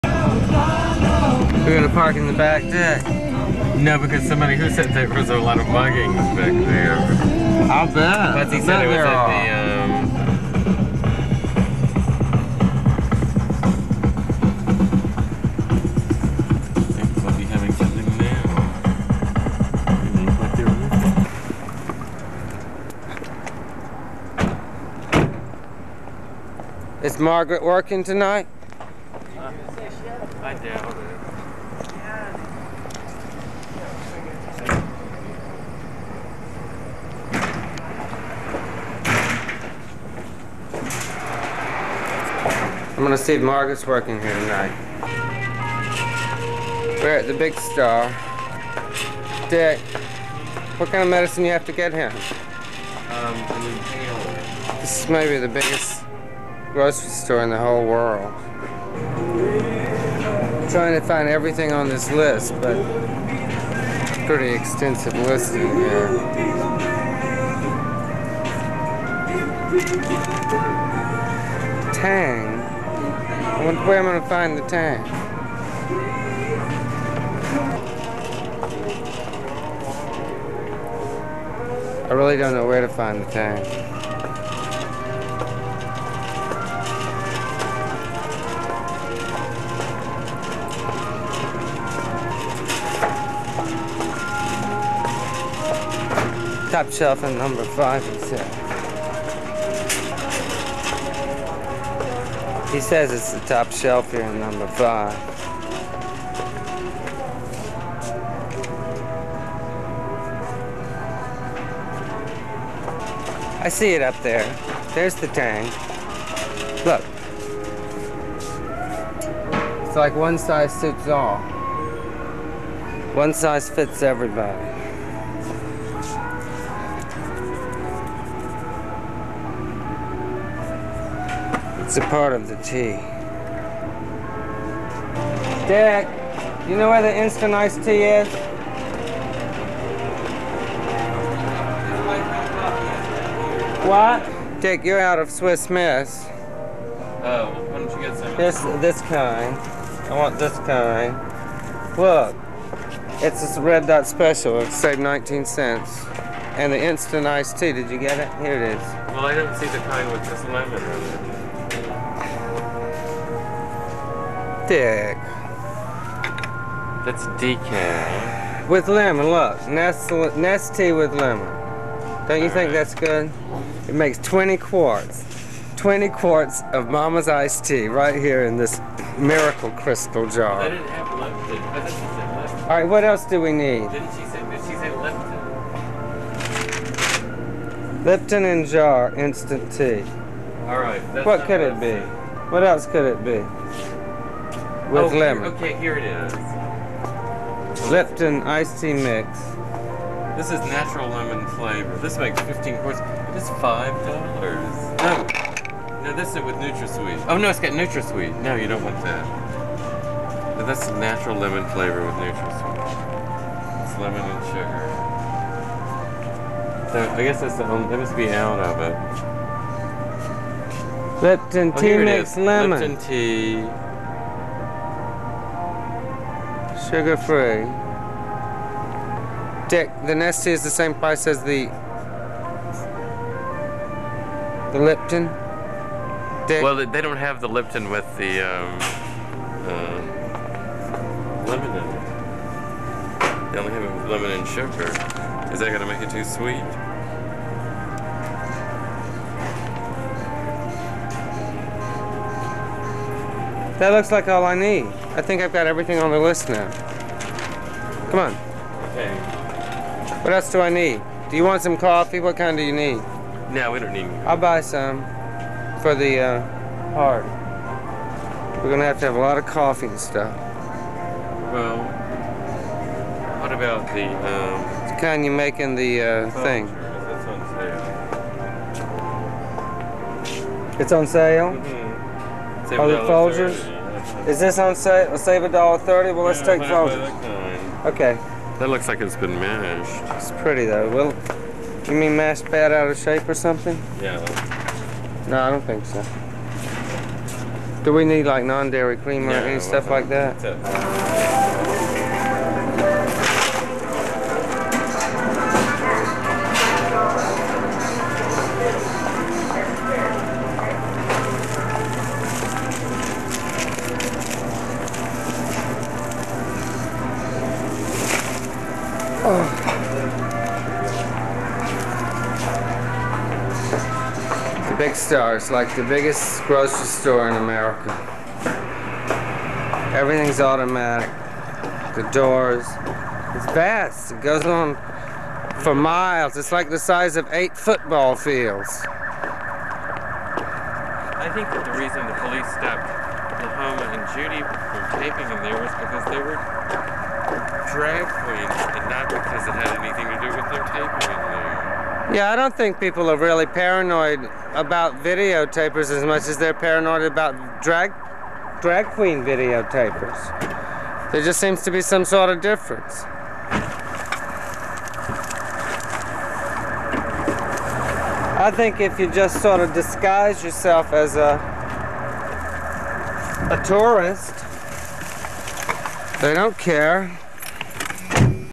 Nelson Goes to a Grocery Store in Atlanta in 1989
While visiting Atlanta, Nelson goes to the grocery store to get the ingredients for his mother's spice tea that he wants to seve at a party he's hosting after RuPaul's nightclub show later that week.  Video by Nelson Sullivan